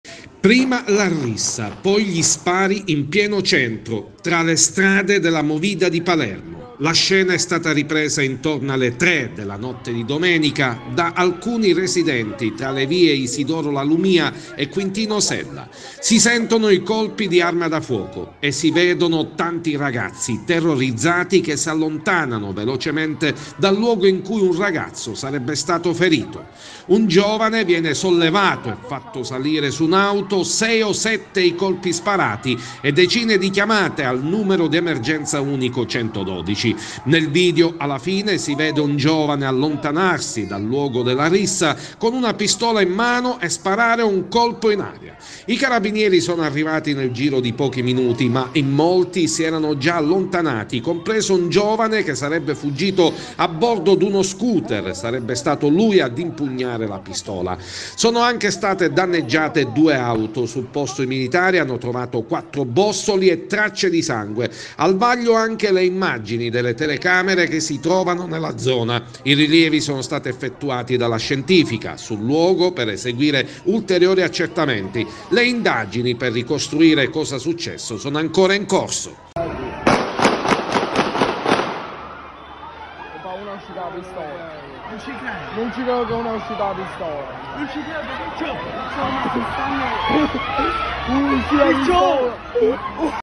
Malamovida a Palermo a colpi di pistola [STUDIO 98]
A Palermo si è scatenata una maxi rissa con colpi d’arma da fuoco esplosi tra via Quintino Sella e via Isidoro La Lumia, intorno alle 3 della notte tra sabato e domenica. Numerosi residenti hanno udito distintamente sei o sette colpi d’arma da fuoco in strada esplosi da una persona che poi è fuggita a bordo di una moto Honda Sh. Sul posto sono intervenuti i Carabinieri che hanno acquisito i video delle telecamere di sorveglianza nella zona. I militari hanno trovato tracce di sangue.